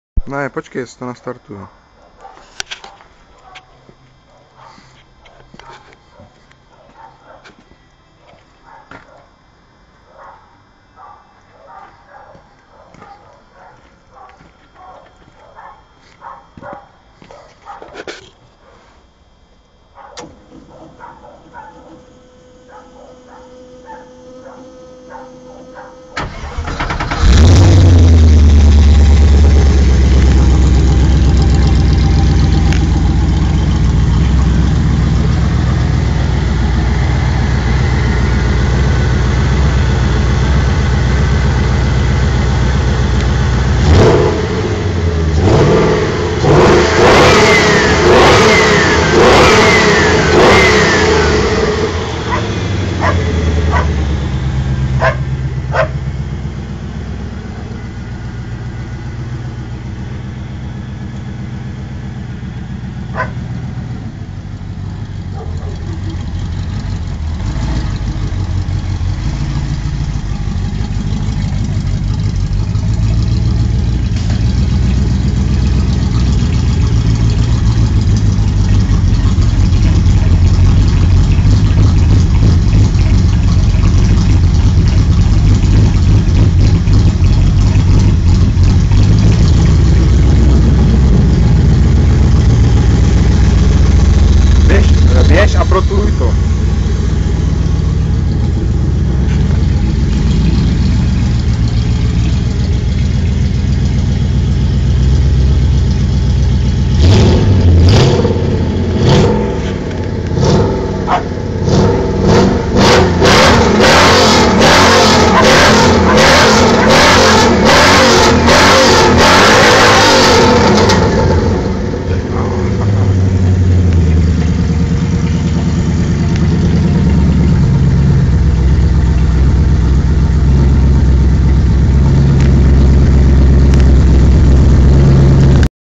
Wings Team - PEPE 2008
Loni pri prvnich testech naseho zavodniho specialu 4.2 V8